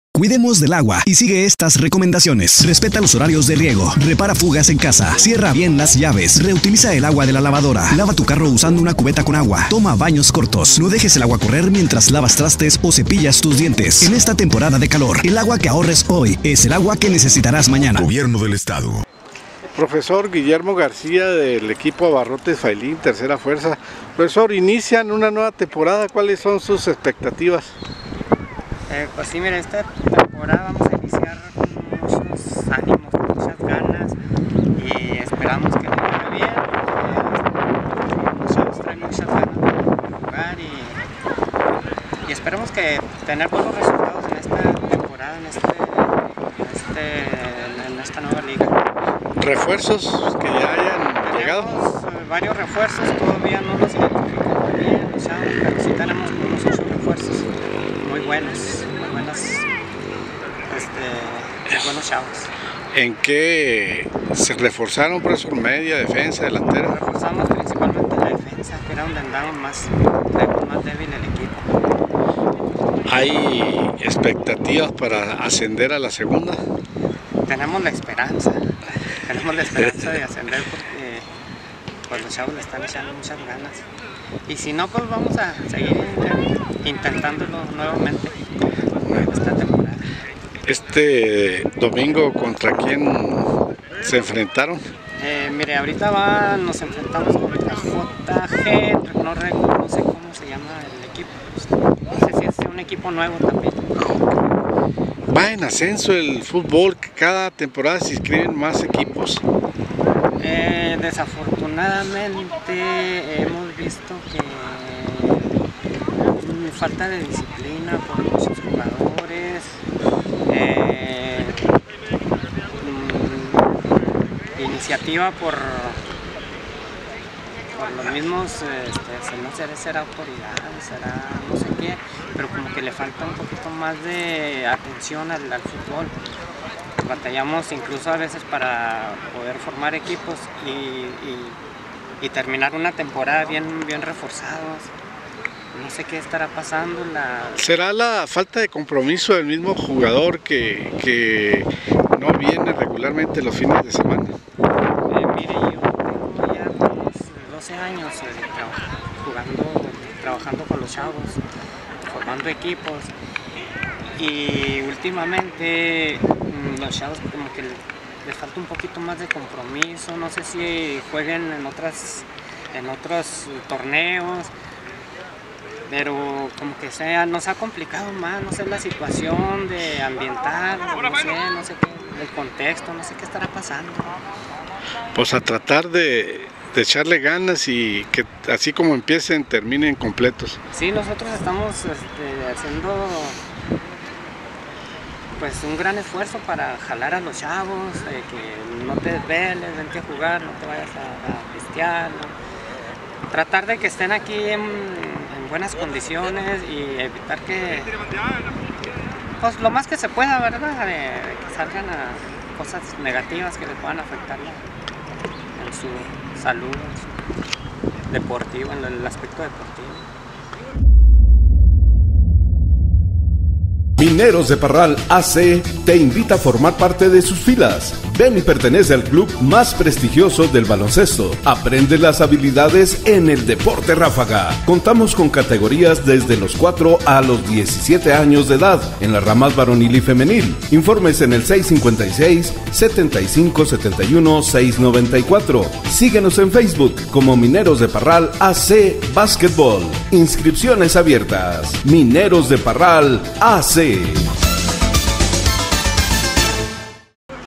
Guillermo García
Guillermo García de Deportivo Saidy inician una nueva etapa, una nueva temporada n el Torneo Juan Chanate Ramírez